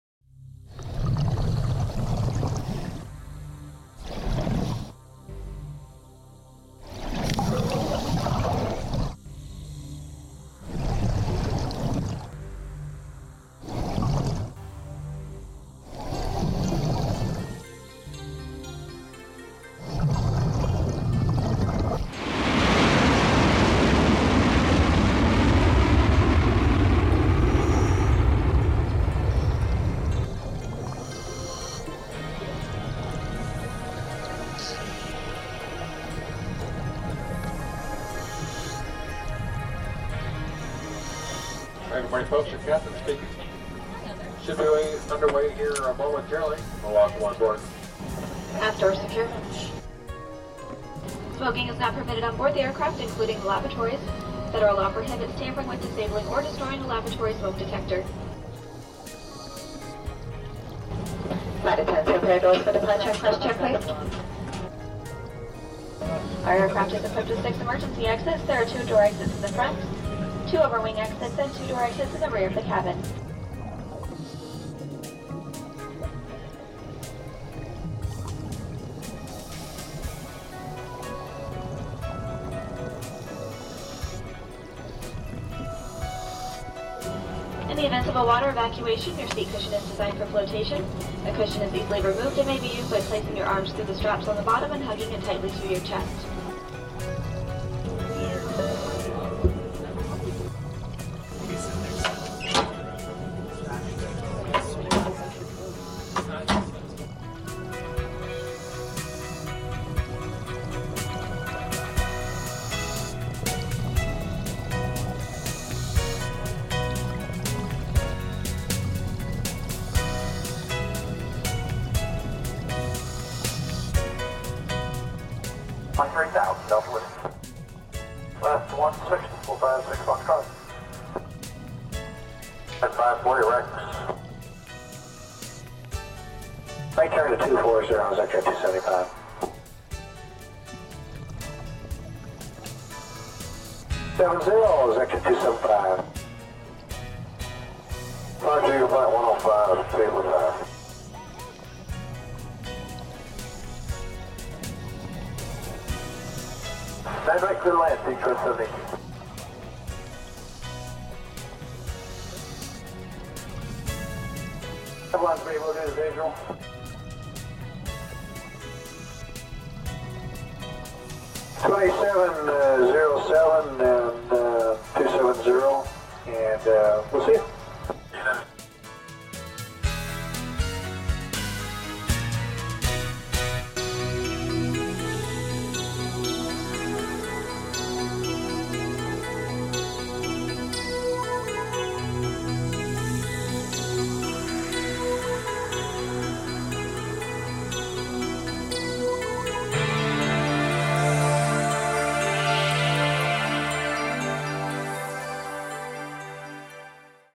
737 Artifical Reef, Music video
Scuba Dive a 737 Jet. B.C. Canada.Underwater video.